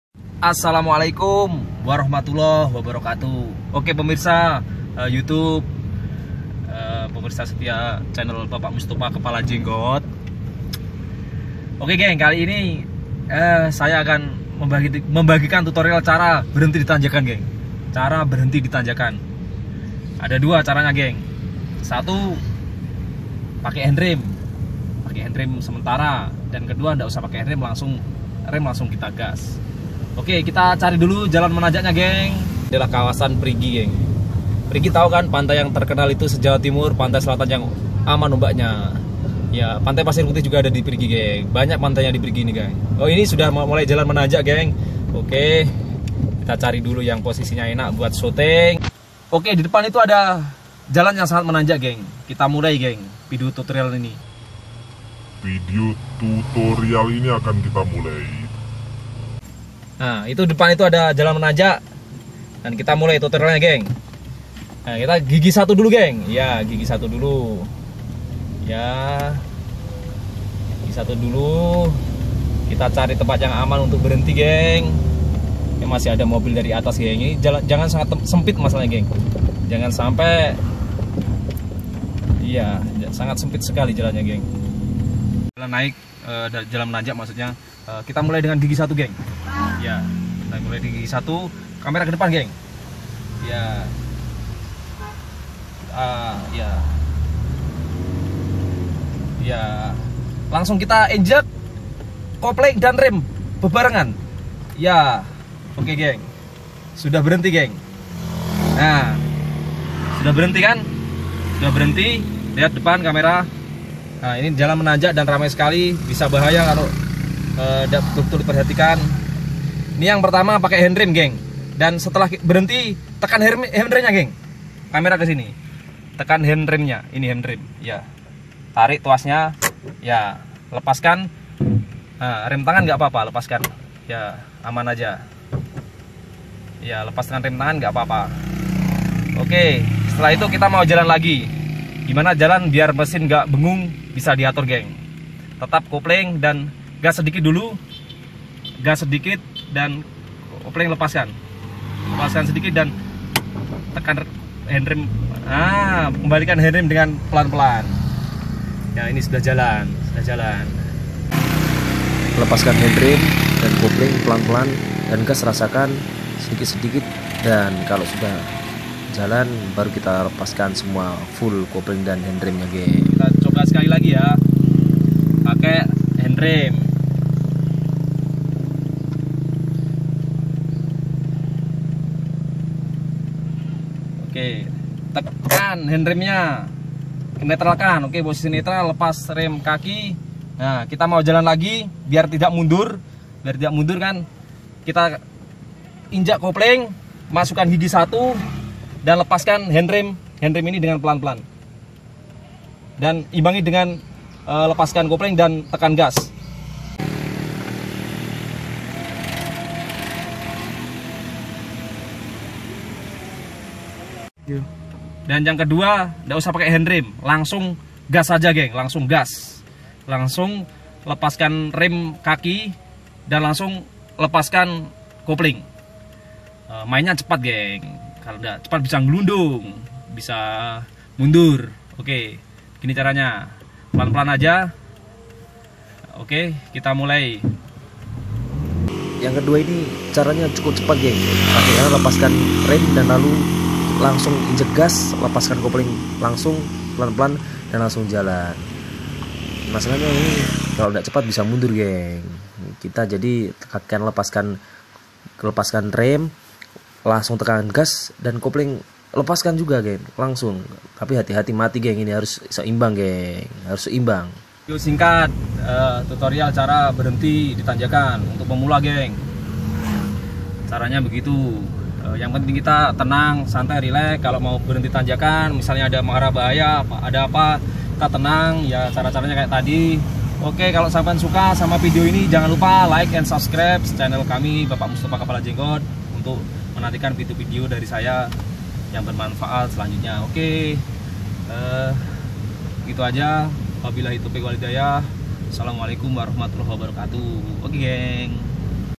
CARA BERHENTI SEBENTAR DI TANJAKAN AGAR TIDAK MUNDUR/MESIN TIDAK MATI | TUTORIAL MENGEMUDI MOBIL # 5
Video ini adalah trik cara mengemudi mobil di tanjakan berhenti sebentar dan jalan lagi agar mesin tidak mati dan tidak mundur